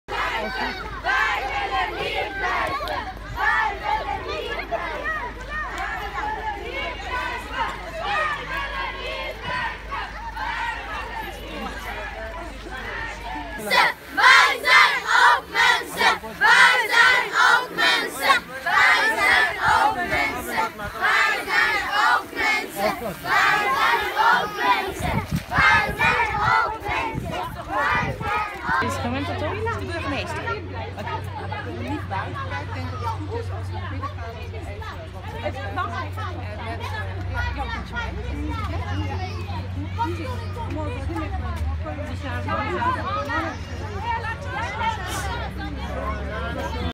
Asielzoekers in protest om uitzettingsbeleid
Een groep van zo'n zestig asielzoekers toog vrijdagochtend naar het gemeentehuis in Burgum. Ze vragen aandacht voor het feit dat ze in angst leven omdat ze elk moment uitgezet kunnen worden. Vrijdagochtend kwam ook de vreemdelingenpolitie langs om een man uit te zetten. Deze zou vervolgens van de trap zijn gevallen waarna een ambulance en de traumahelikopter ter plaatse moesten komen.